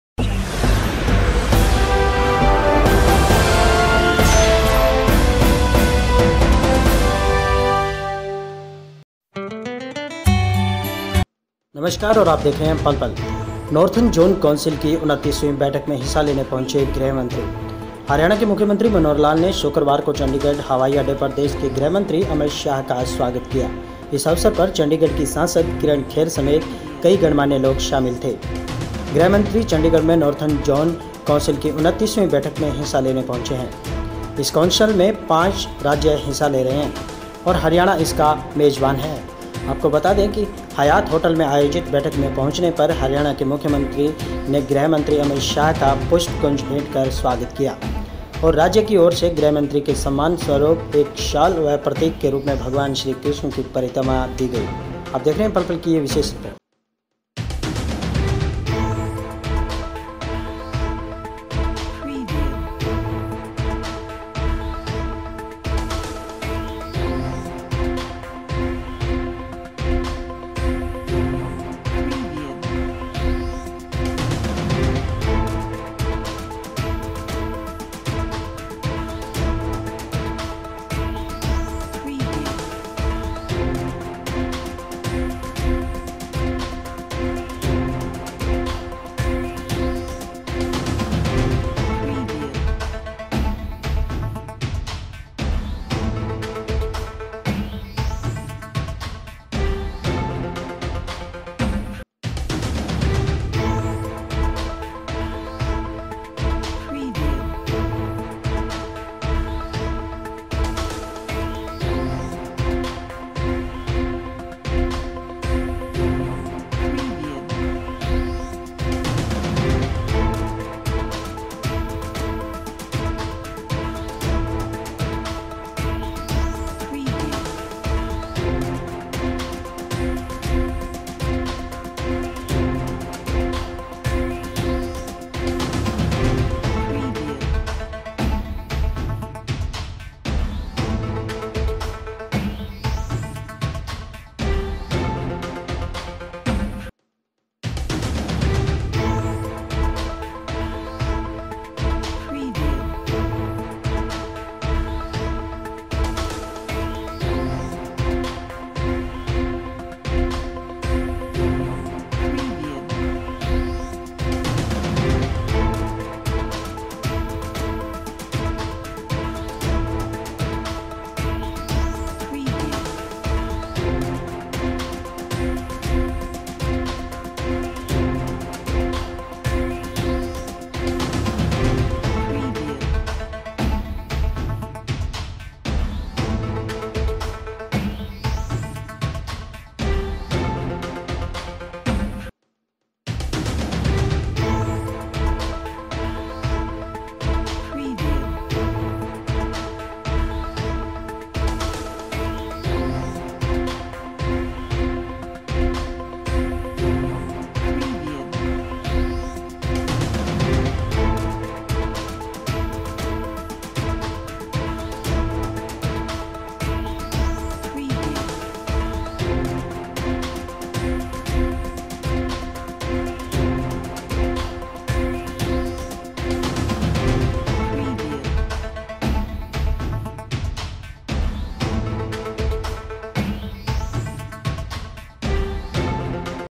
चंडीगढ़ की सांसद किरण खेर समेत cm मनोहर लाल खट्टर ने किया स्वागत#
amitshah का चंडीगढ़ की सांसदकिरणखेर# समेत cm मनोहर लाल खट्टर ने किया
हरियाणा के मुख्यमंत्री मनोहर लाल ने शुक्रवार को चंडीगढ़ हवाई अड्डे पर देश के गृह मंत्री अमित शाह का स्वागत किया। इस अवसर पर चंडीगढ़ की सांसद किरण खेर समेत कई गणमान्य लोग शामिल थे। गृह मंत्री चण्डीगढ़ में नॉर्थरन जॉन कौंसिल की 29वीं बैठक में हिस्सा लेने पहुंचे हैं। इस कौंसिल में 5 राज्य हिस्सा ले रहे हैं और हरियाणा इसका मेजबान है। हयात होटल में आयोजित बैठक में पहुंचने पर हरियाणा के मुख्यमंत्री ने  गृह मंत्री अमित शाह  को पुष्प गुच्छ भेंट कर स्वागत किया और राज्य की ओर से गृह मंत्री को सम्मान स्वरूप एक शॉल एवं प्रतीक के रूप में भगवान श्रीकृष्ण की प्रतिमा दी गई। नॉर्थरन जॉन कौंसिल की 29वीं बैठक बैठक जारी है।